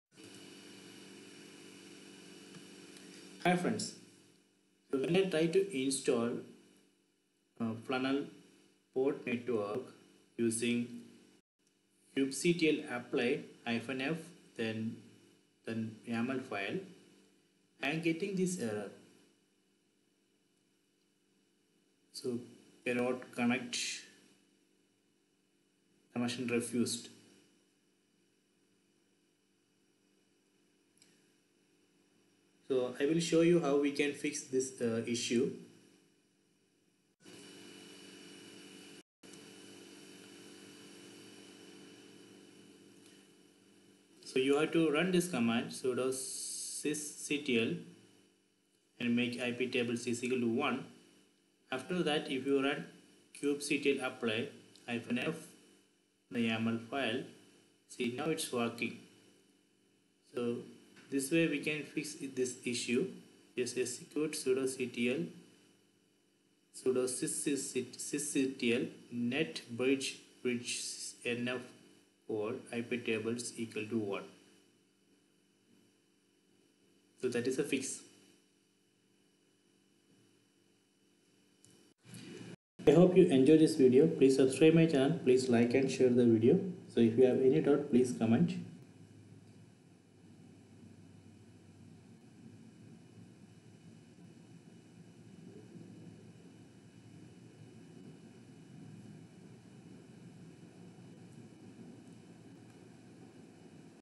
Kubernetes kubectl dial tcp 127.0.0.1:8080: connect: connection refused
sudo kubectl apply -f
dial tcp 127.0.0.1:8080: connect: connection refused